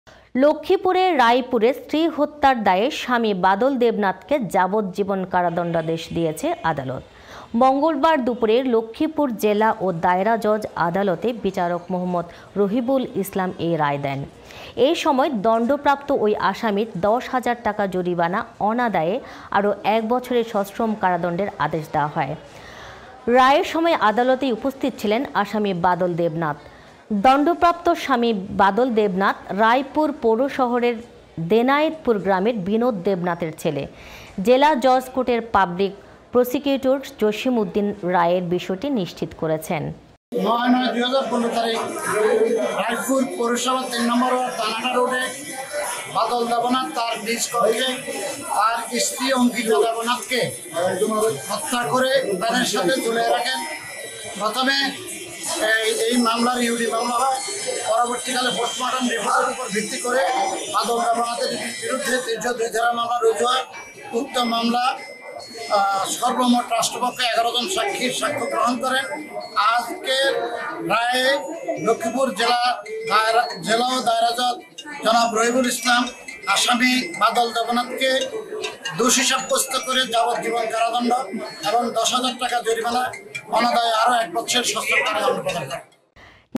লক্ষ্মীপুরের রায়পুরে স্ত্রী হত্যার দায়ে স্বামী বাদল দেব নাথকে যাবজ্জীবন কারাদণ্ডাদেশ দিয়েছেআদালত।
লক্ষ্মীপুরের রায়পুরে স্ত্রী হত্যার দায়ে স্বামী বাদল দেব নাথকে যাবজ্জীবন কারাদণ্ডাদেশ দিয়েছে
আদালত। মঙ্গলবার দুপুরে লক্ষ্মীপুর জেলা ও দায়রা জজ আদালতের বিচারক মো. রহিবুল ইসলাম এ রায় দেন। এ সময়
দণ্ডপ্রাপ্ত ওই আসামীর ১০ হাজার টাকা জরিমানা অনাদায়ে আরো ১ বছরের সশ্রম কারাদন্ডের আদেশ দেয়া হয়। রায়ের
সময় আদালতেই উপস্থিত ছিলো আসামী বাদল দেব নাথ। দন্ডপ্রাপ্ত স্বামী বাদল দেব নাথ রায়পুর পৌর শহরের
দেনায়েতপুর গ্রামের বিনোদ দেবনাথের ছেলে। জেলা জজ কোর্টের পাবলিক প্রসিউকিউটর জসিম উদ্দিন রায়ের বিষয়টি
নিশ্চিত করেছেন।

সিংক ঃ এ্যাডভোকেট জসিম উদ্দিন, পাবলিক প্রসিউকিউটর, জেলা জজ আদালত, লক্ষ্মীপুর।
iON TV
SKY 782